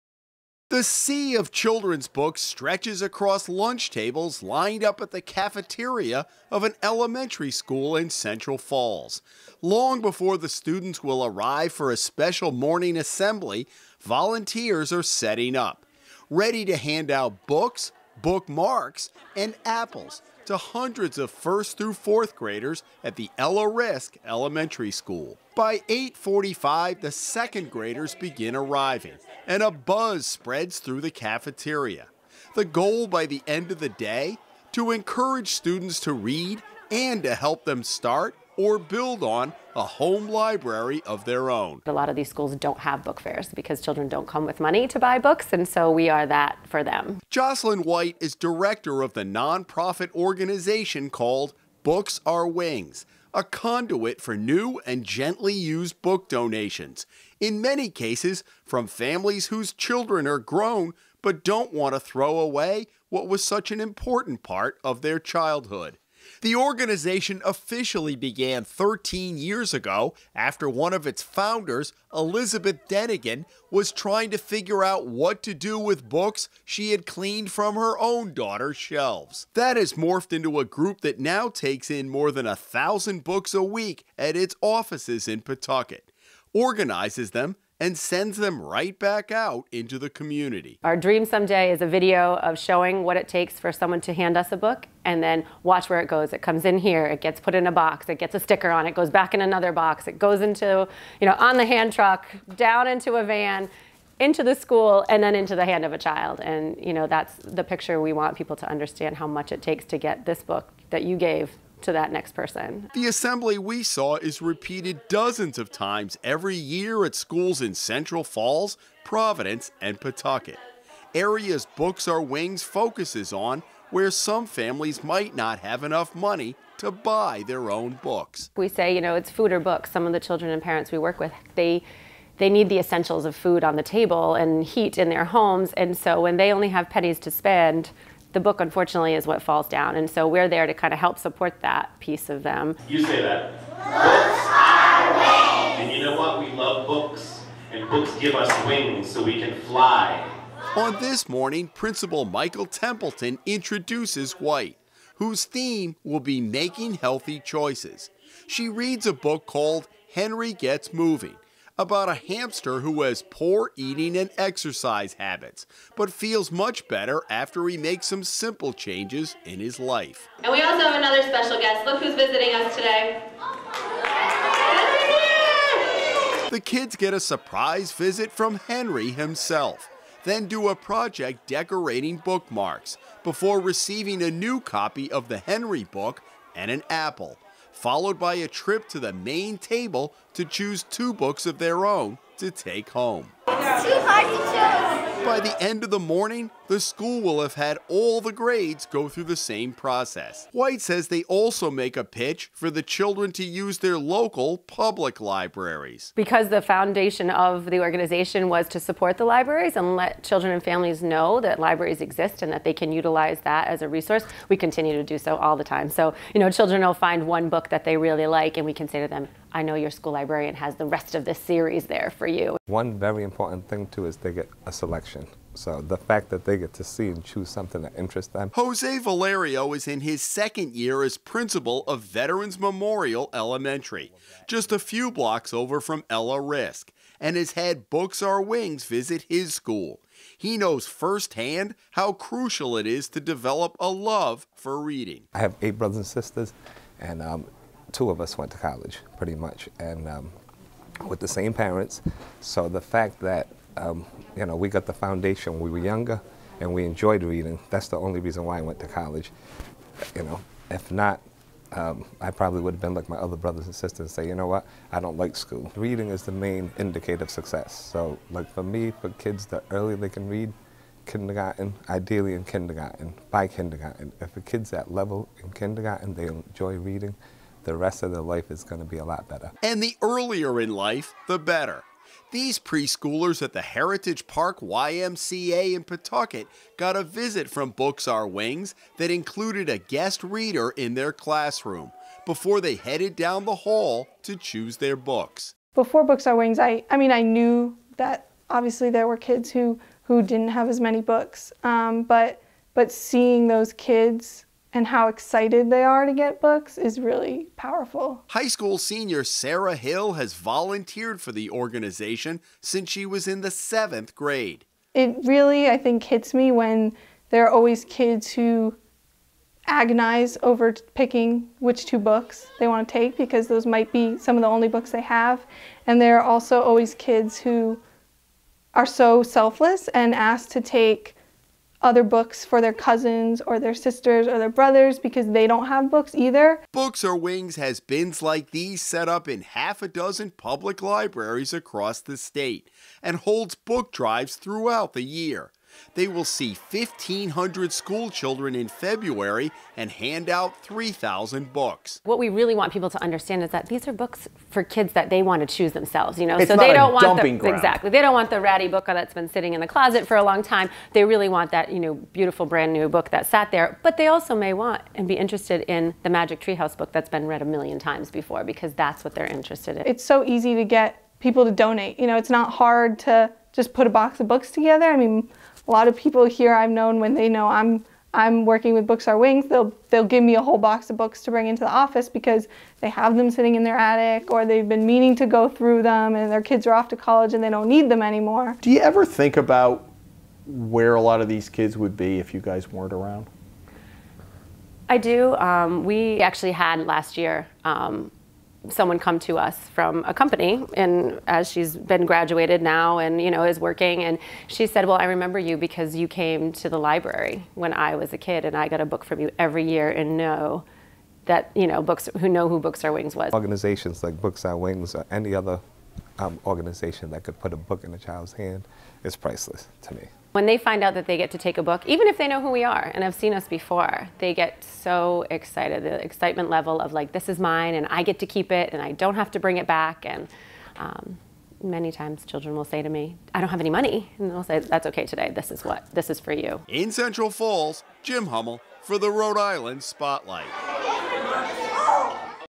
A Book Of Their Own 2.16.2017
Every year tens of thousands of children’s books make their way into the Rhode Island homes of students whose families might not be able to afford starting a home library of their own. For more than a decade Books Are Wings has provided an outlet for families looking for a way to donate their own children’s books and help other students at the same time. This month Jim Hummel takes a look at what it take to get from start to finish.